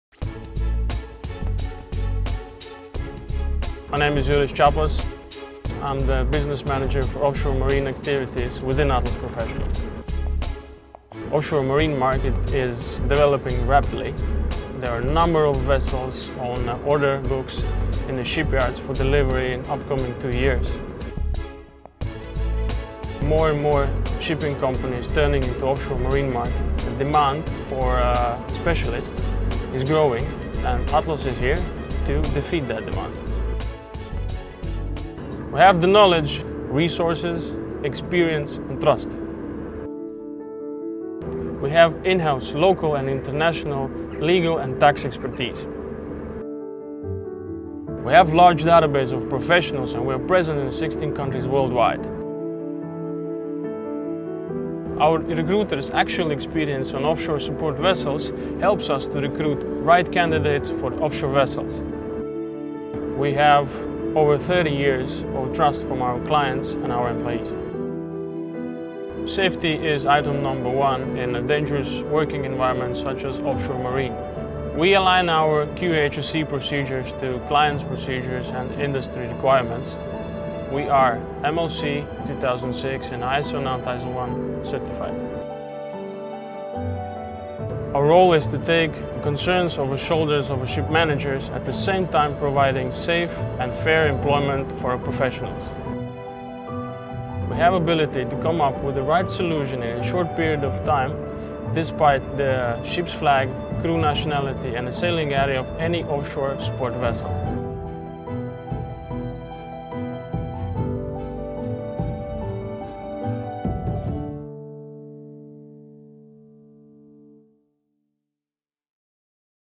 Atlas in the Offshore Marine Industry | Atlas Professionals
Juris Capass, Business Manager for Offshore Marine activities within Atlas Professionals, provides an insight into what Atlas Professionals can do to meet the demands of the Offshore Marine industry.